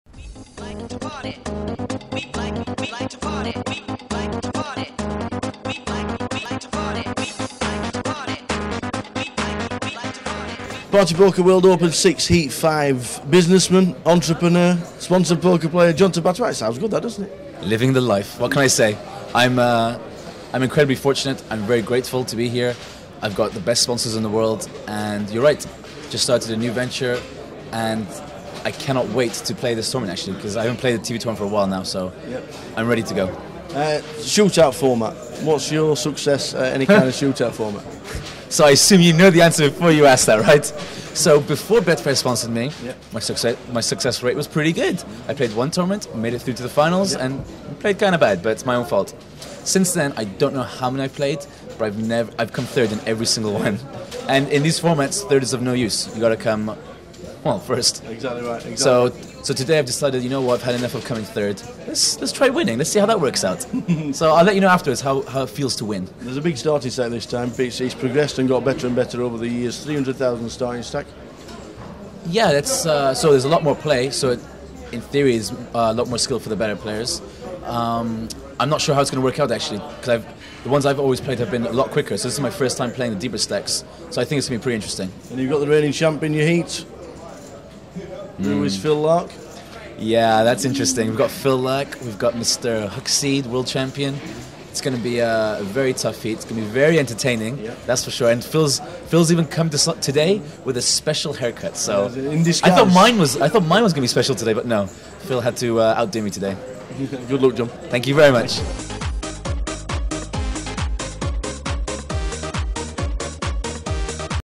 Party Poker World Open VI - John Tabatabai
We speak to John Tabatabai who is looking forward to todays heat.